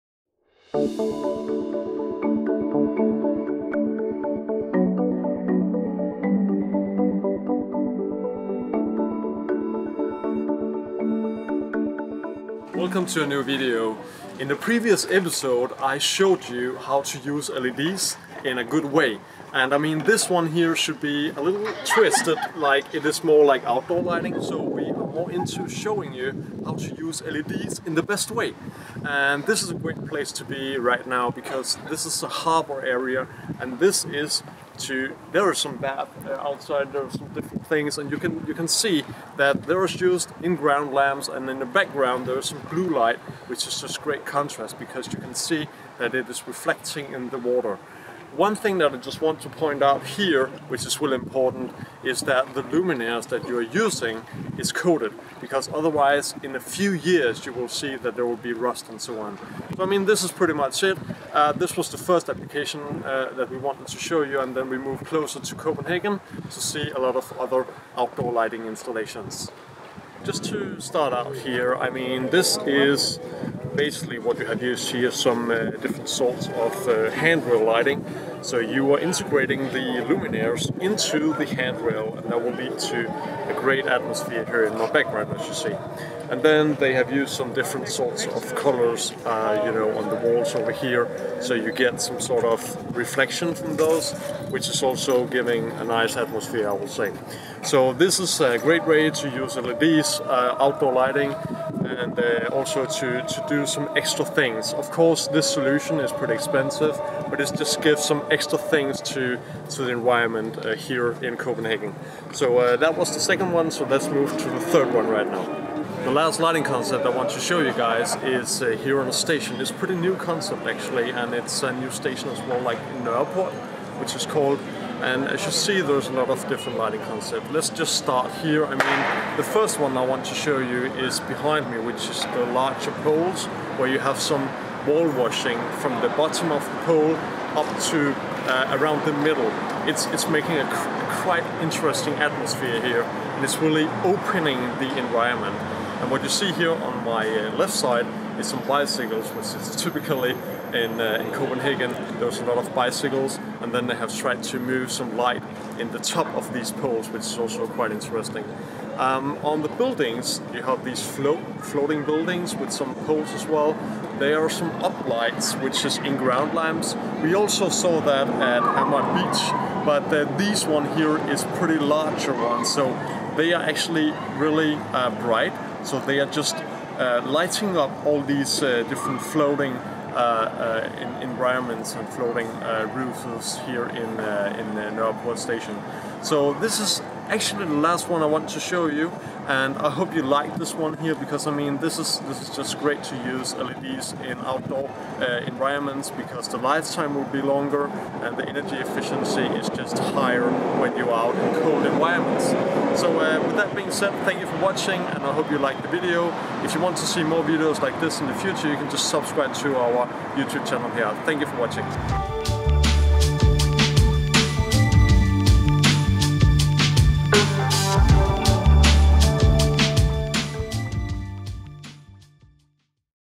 Where to use LEDs (Copenhagen) #002
A short video on how to use LEDs. This video includes different applications mainly in Copenhagen.